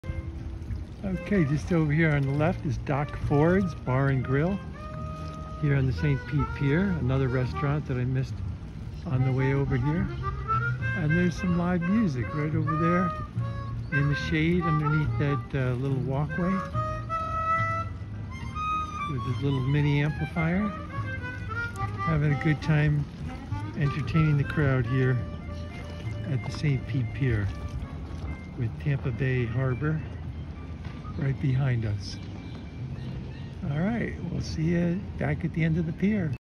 Live Music at the St Pete Pier
A hot day on the pier, even in the shade!